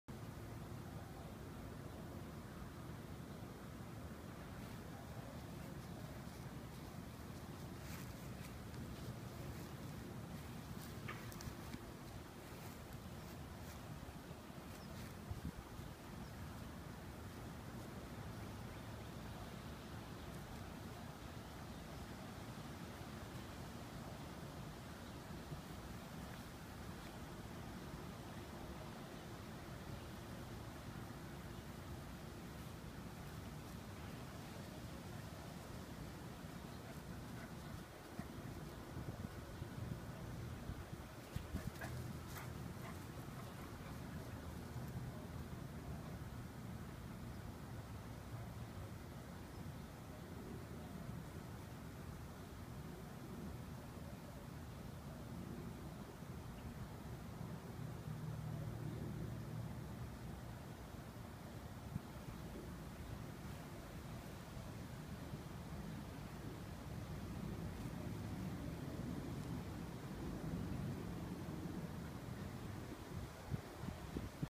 Dasher and The Door Mat
The door mat has been placed in a patio chair, in the yard, in the sun... to dry. Watch Dasher figure out if the mat is friend or foe!